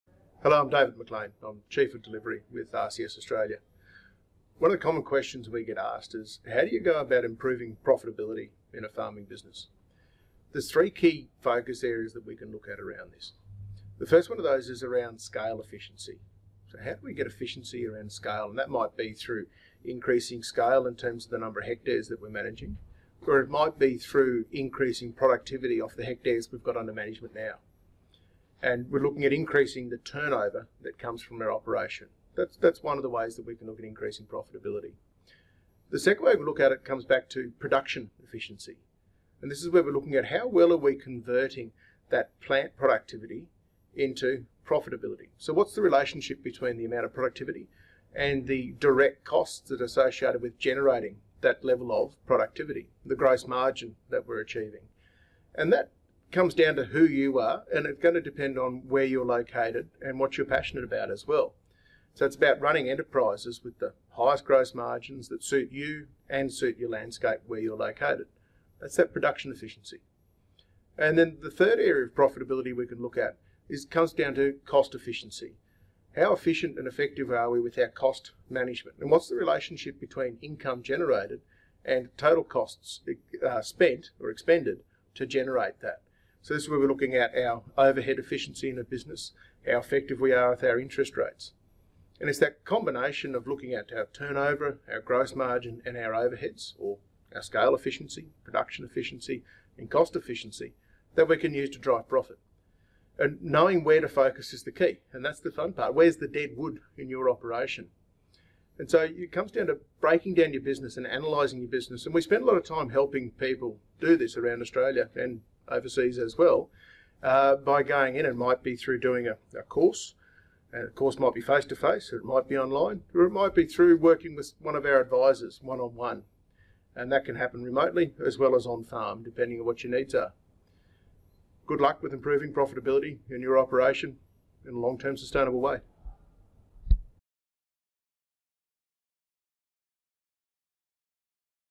‘How can farming businesses improve profitability?' with RCS Australia's David McLean
This week, QFF took time to catch up with our partner RCS Australia, leading provider of regenerative agriculture education and advise, to discuss the important topic of ‘How can farming businesses improve profitability?’. In this short video, RCS Australia’s Chief of Delivery, David McLean, talks through the three key focus areas farming business can look at to increase profitability including scaling efficiency, production efficiency and cost efficiency.